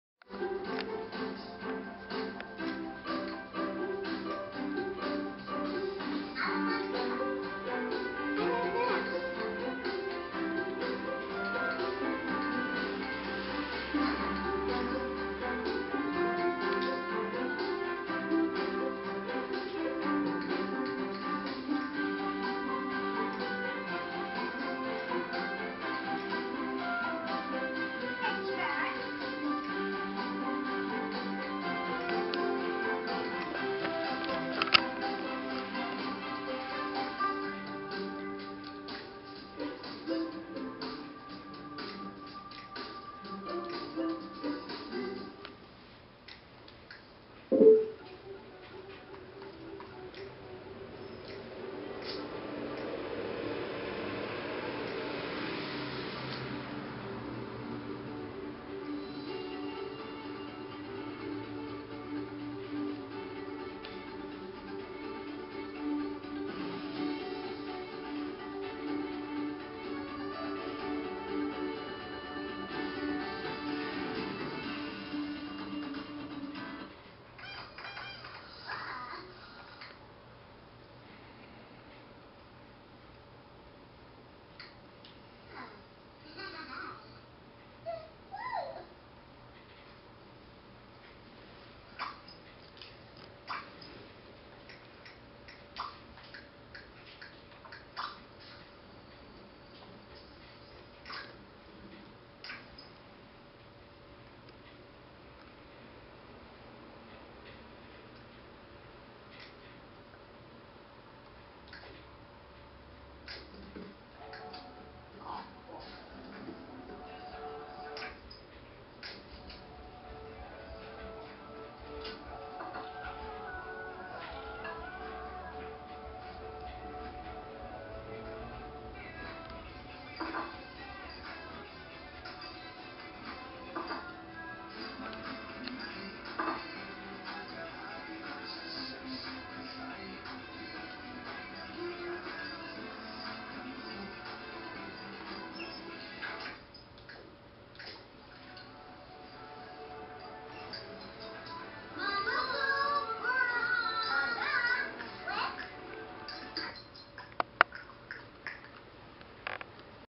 Sparrow 4 weeks
Little Miss Sparrow FoFo at 4 weeks old , cutest sugar glider ever.